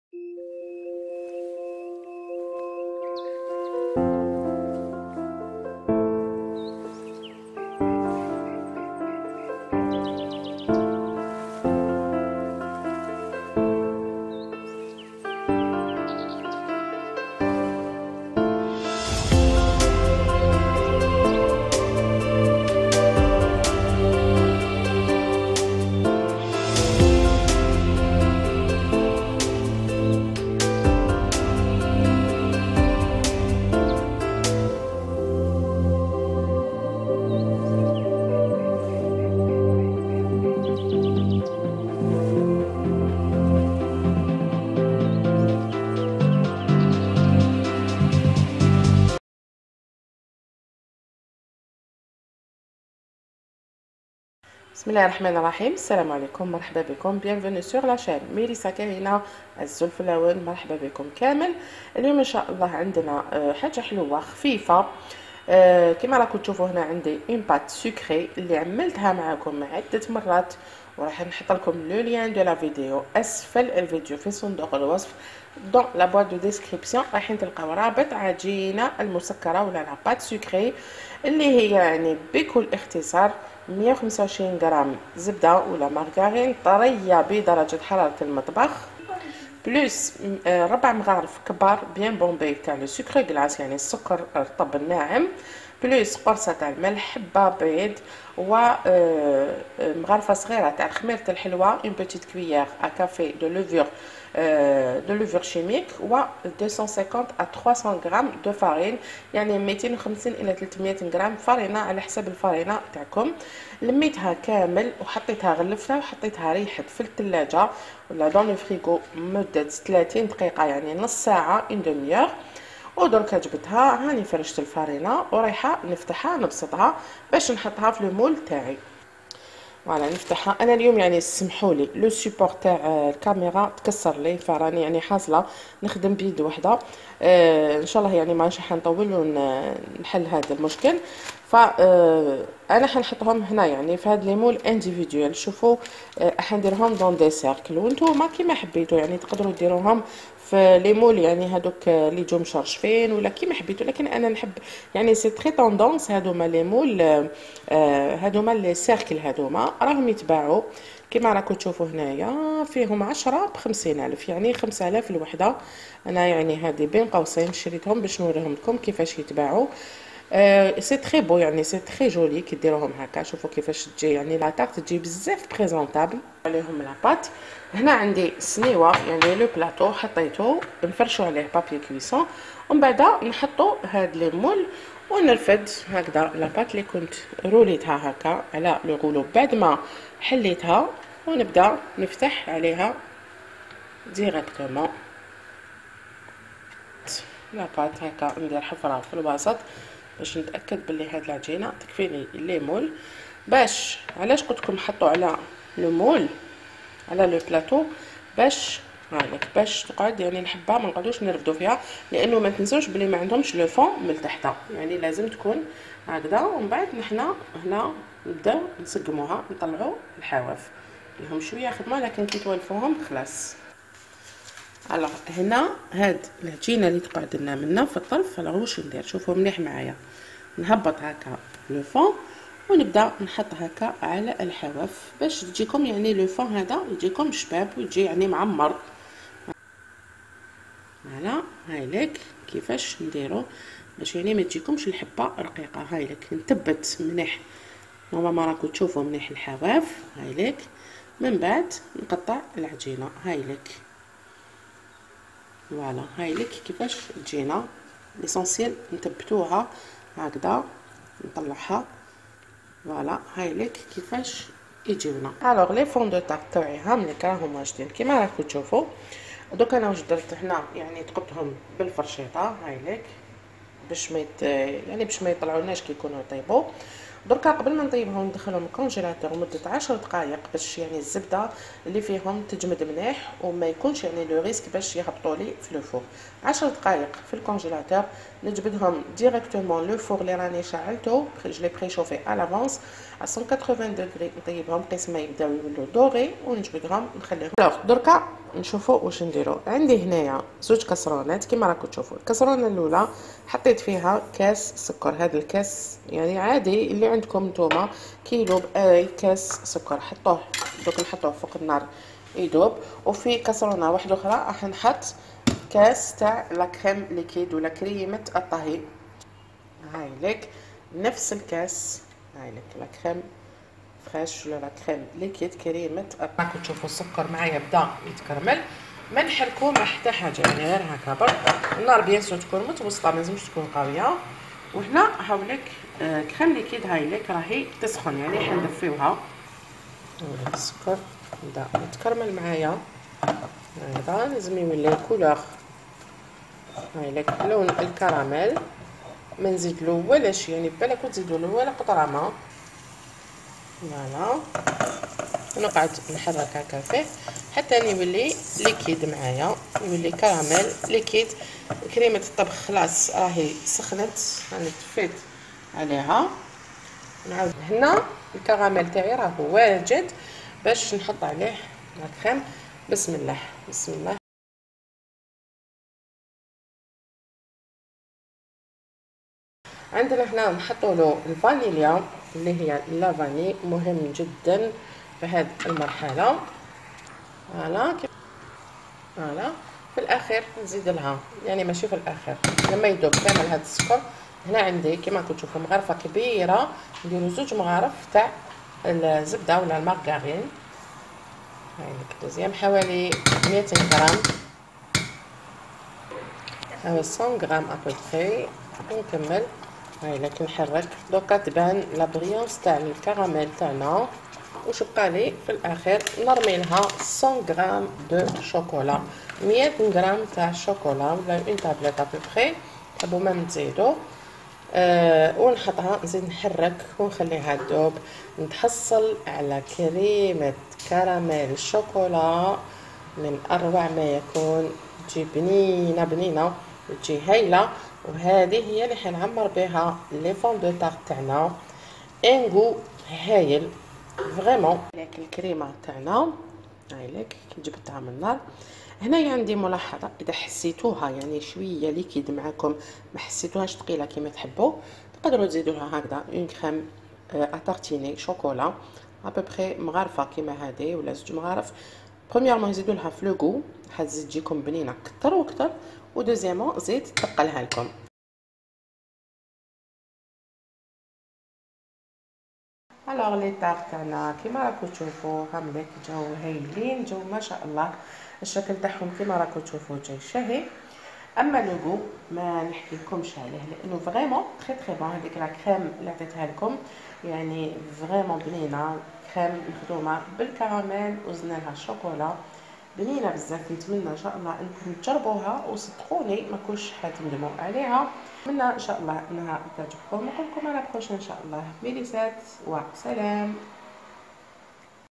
tartes a la créme caramel chocolat تارتولات بكريمة الكراميل والشوكولا
tarte a la créme caramel chocolat trés reussit et facile une tuerie et vrai delice
recette de pate sucrée